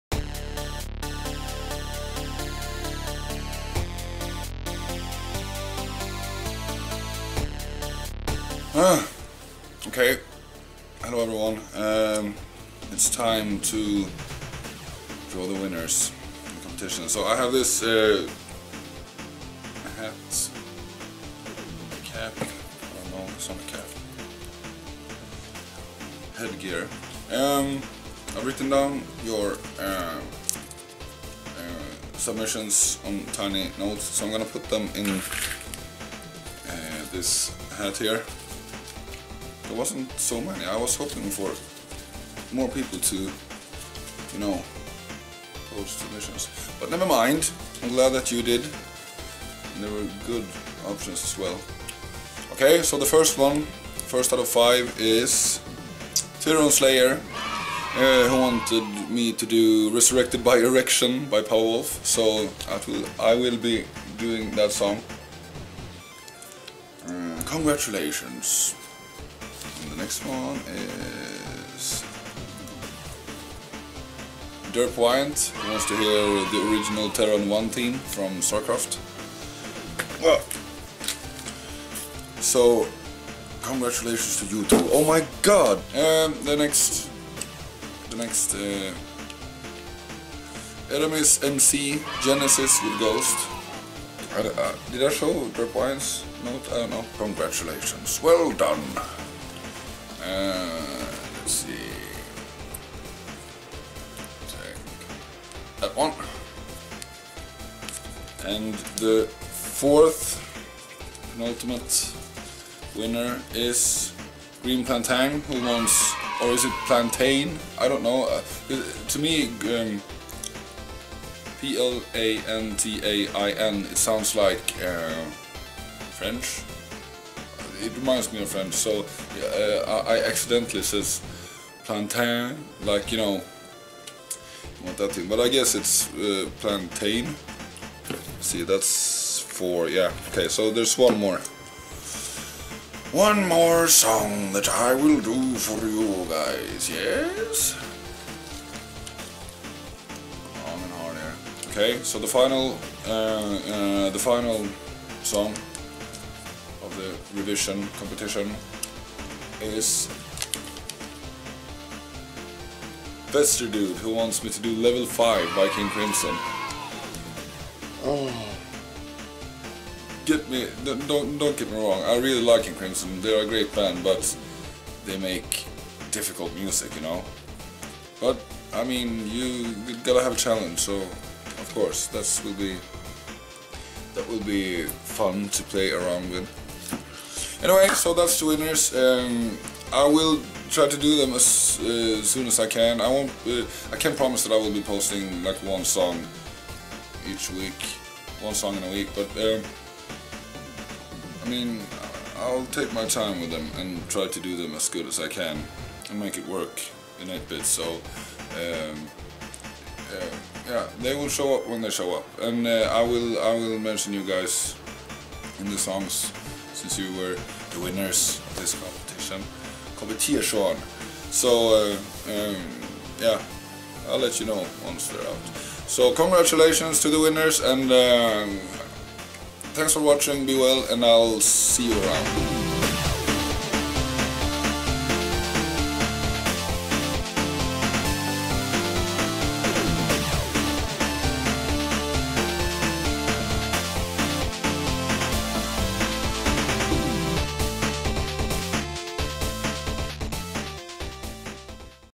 Revision Competition - THE WINNERS
The five winners whose submissions will be done into 8-Bit revisions.
This means that the Meliora project will be put on hold since these songs are now top priority.

With that said, I will continue this lottery every now and then, picking one song that I'll do. This will include ALL the songs you've wished for.